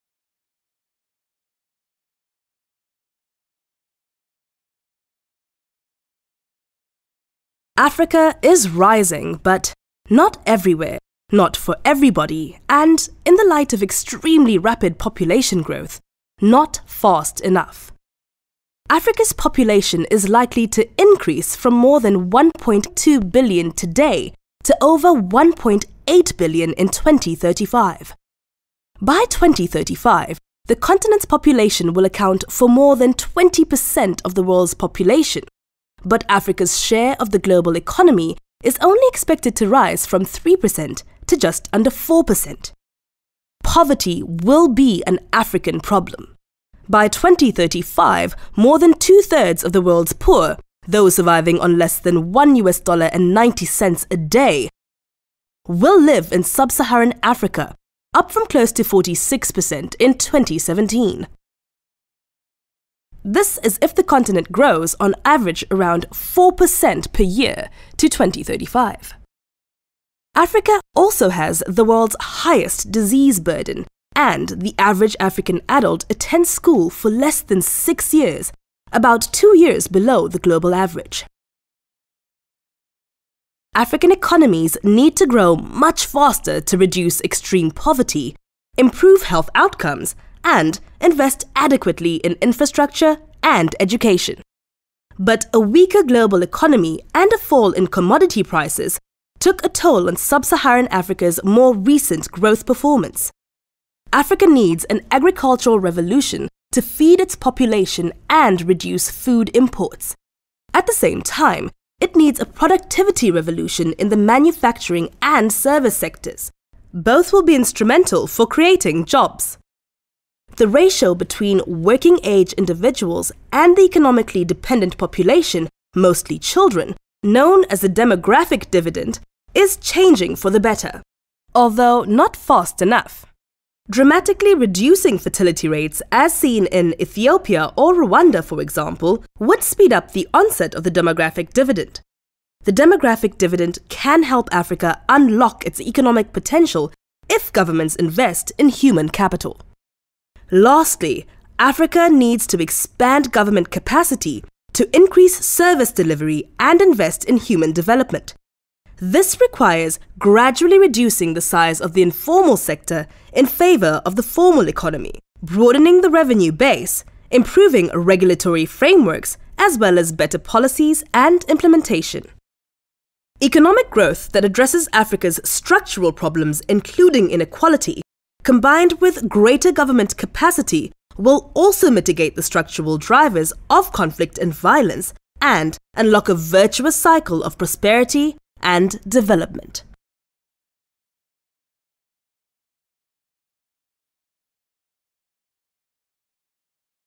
Africa’s future 2035
Africa’s future is promising, but progress must be accelerated. How can this be achieved?